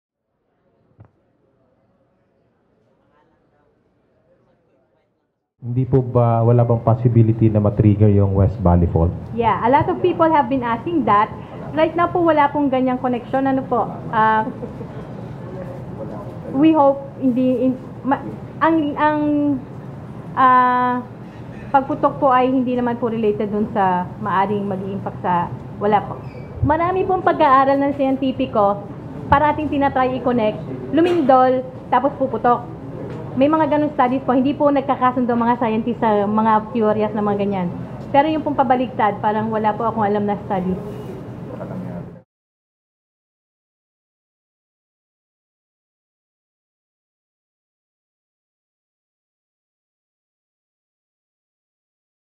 Phivolcs: Taal eruption won’t trigger tremors from West Valley Fault
The Philippine Institute of Volcanology and Seismology (Phivolcs) assured Monday that Taal Volcano’s eruption that causes numerous volcanic earthquakes in Batangas and nearby areas would not cause more tremors coming from the dreaded West Valley Fault.